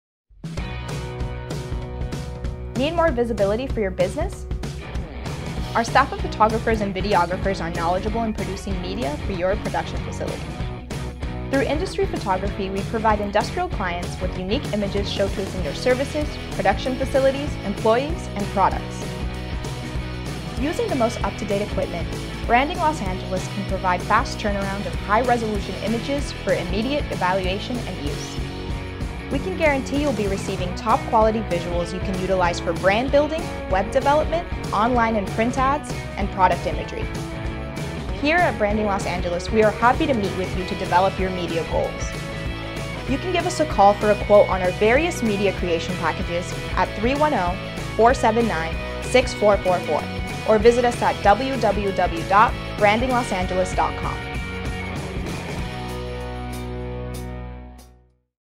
Industrial Photo and Video Services
Through Industry Photography we provide industrial clients with unique images showcasing their services, production facilities, employees and products.